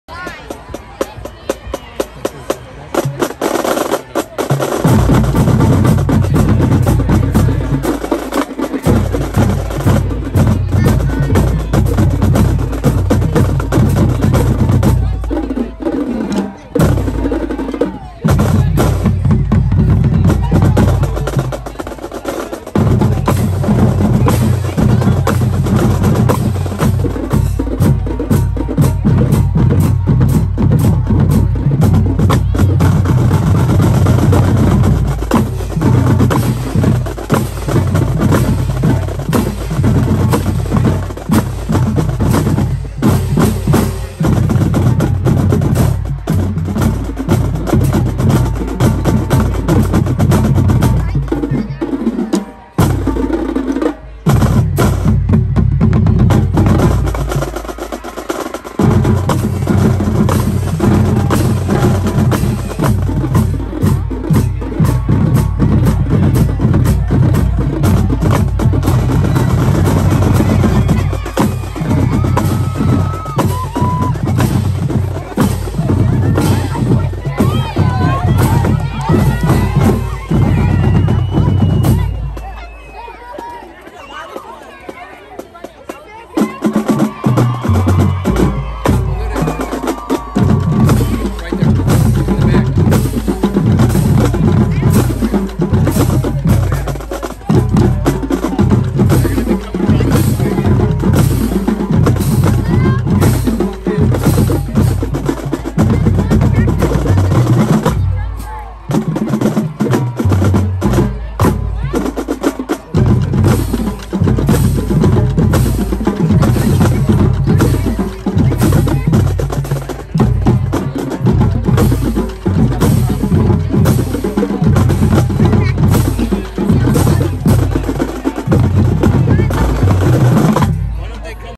Marching Storm Drumline Parade of Athletes 2 - 2021 Taste Under the Lights
Marching Storm Drumline Parade of Athletes 2 - 2021 Taste Under the Lights - 8/20/21 - SEHS Bands - South Elgin High School Marching Storm - Director of Bands: David Grayson